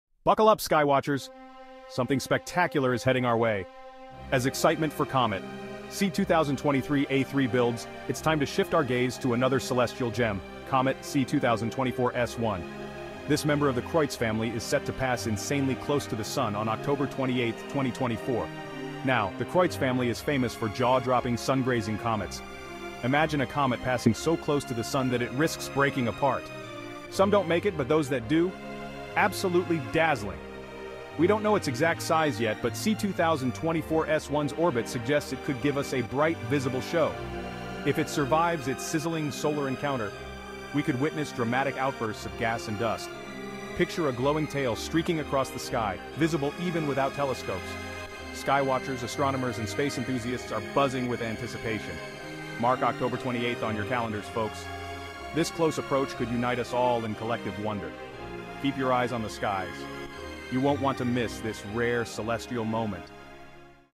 Comet C/2024 S1 Get Ready for a Spectacular Solar Show Oct. 25th-28th
As excitement for Comet C/2023 A3 (Tsuchinshan-ATLAS) begins to build, it’s time to shift our gaze toward another promising celestial event: the approach of Comet C/2024 S1, a member of the Kreutz family of comets. This new visitor, set to pass particularly close to the Sun on October 28th, 2024, has already garnered interest due to its orbit and the potential for a spectacular show as it grazes the solar corona.

The Kreutz family of comets is famous for producing some of the most stunning sungrazing comets in history. These comets often pass perilously close to the Sun, and while many break apart due to the intense heat and gravitational forces, others have produced dazzling displays visible even in daylight. While the size of C/2024 S1 remains unknown, its orbit suggests it could come exceptionally close to the Sun, making this a prime candidate for a bright, visible comet as it makes its passage.

With Comet C/2024 S1 approaching the Sun on October 28th, there is a growing sense of anticipation about what this close encounter could reveal. If the comet survives its solar encounter, it could offer skywatchers a rare spectacle, visible in the early morning or just after sunset depending on its behavior. The intense proximity to the Sun could cause dramatic outbursts of gas and dust, creating a bright, visible tail that could be observed even without telescopes, much like previous Kreutz comets.

For those eager to witness this event, it’s an exciting time to follow its progress. Skywatchers, astronomers, and enthusiasts alike are already preparing, knowing that comets from the Kreutz family have the potential to surprise and awe. As October 28th approaches, keep an eye on the skies—this could be one of those rare celestial moments that unites both professionals and the public in collective wonder.